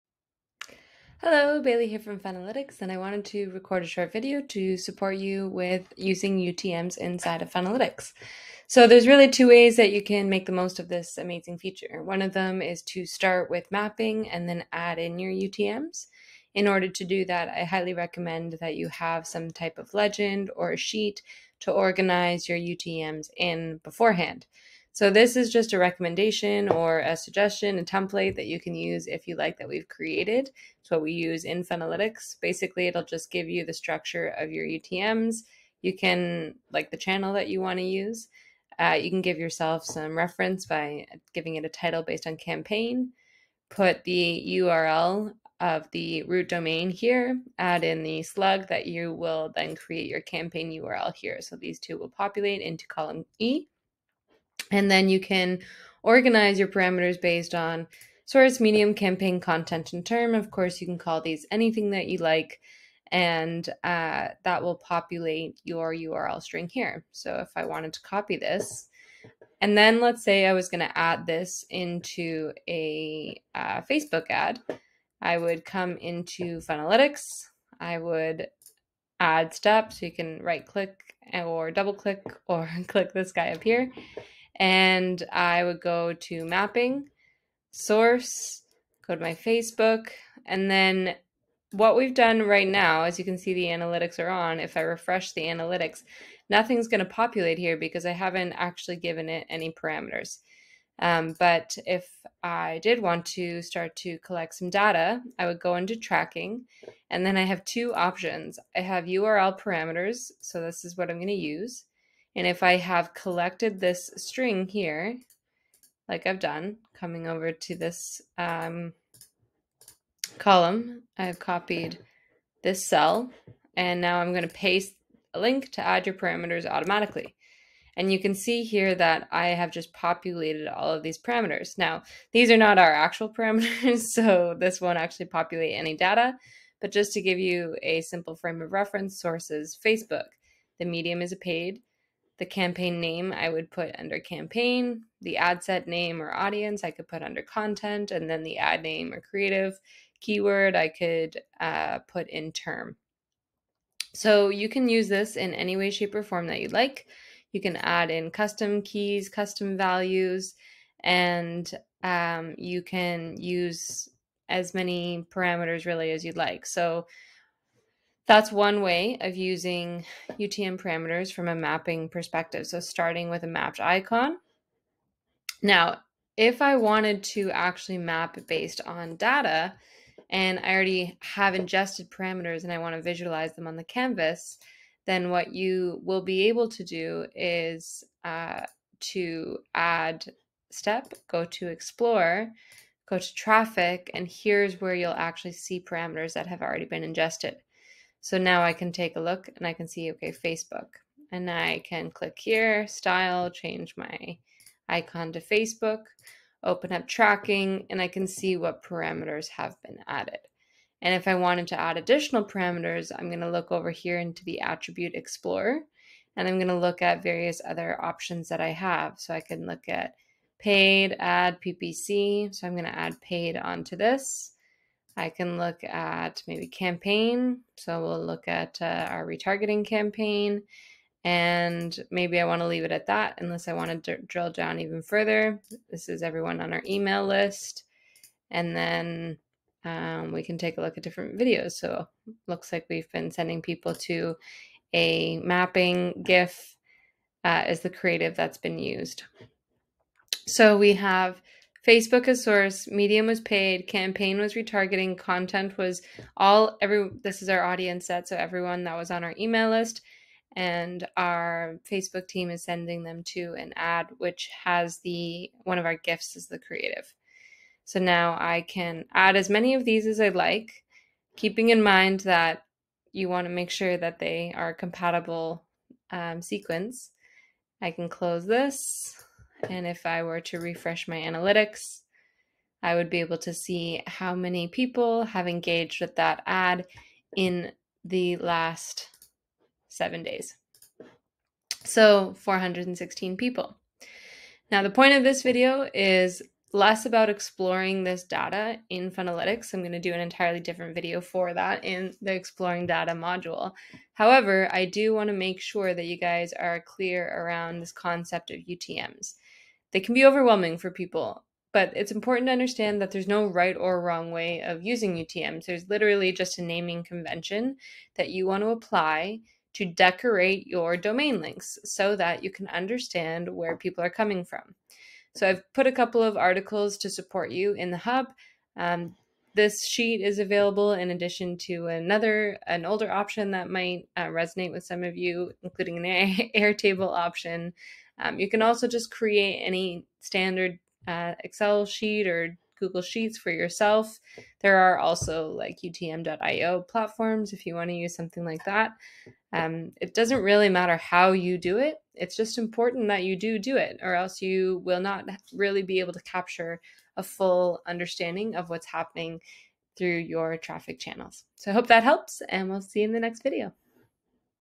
How to track using UTMs inside of Funnelytics
This video is to support you in using UTMs inside of Funnelytics to track, measure and optimize your customer journeys. Baillie, Head of Success at Funnelytics, walks you through two different ways you can achieve this on your canvas.